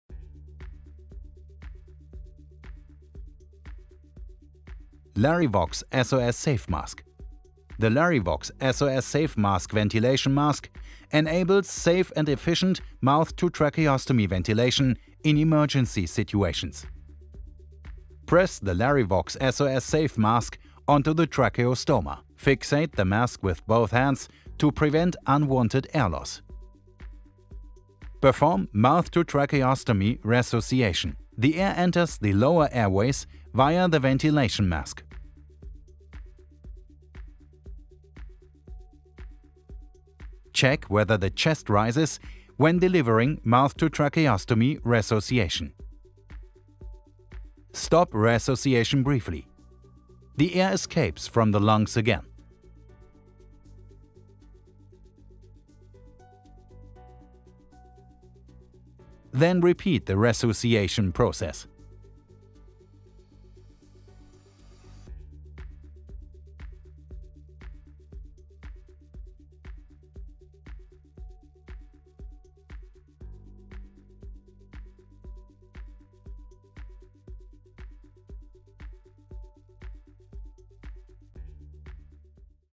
Mouth to tracheostomy ventilation in an emergency | Animation | Fahl Medizintechnik-Vertrieb GmbH
In this animation we explain how to perform a rescue breathing with the LARYVOX® SOS SAFE-MASK in an emergency situation.